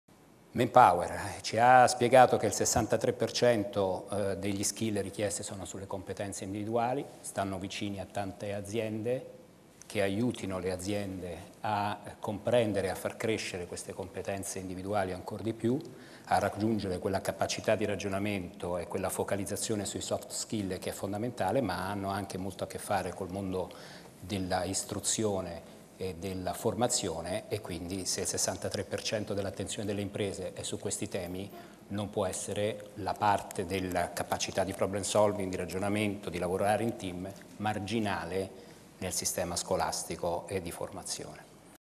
POGGI (DELOITTE): NELLE SCUOLE PUNTARE SUL LAVORO IN TEAM
(Agenzia VISTA) - Roma, 10 Novembre 2015 - L' indagine demoscopica condotta da Deloitte e presentata oggi alla Camera dei Deputati durante il "Deloitte Strategy Council 2015".